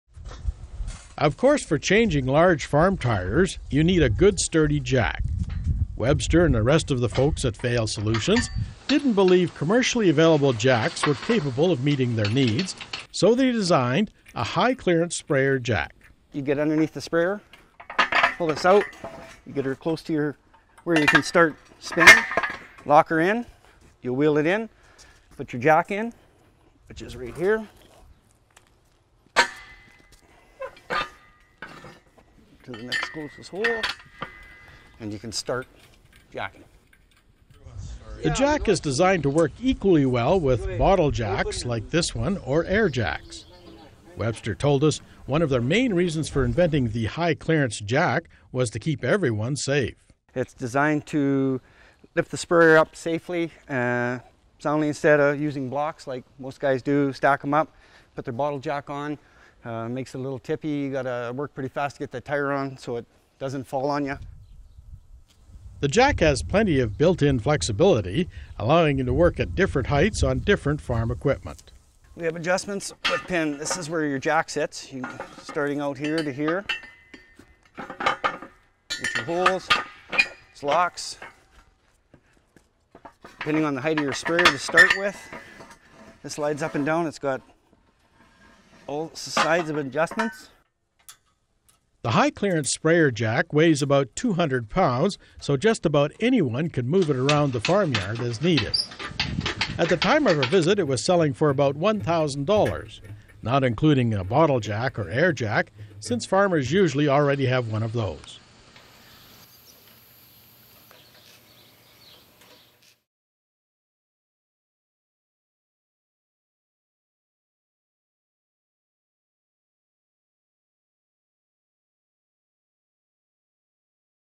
High-Clearance Sprayer Jack
Features: Vale Solutions a farm based manufacturer near Indian Head, Sk., has built an over-sized, heavy duty, fully adjustable jack for high clearance sprayers. The over-sized jack can utilize a bottle jack or an air jack for lifting. It was designed for safer implement tire changes. At the time of our visit to Vale Solutions, it was selling for around $1000.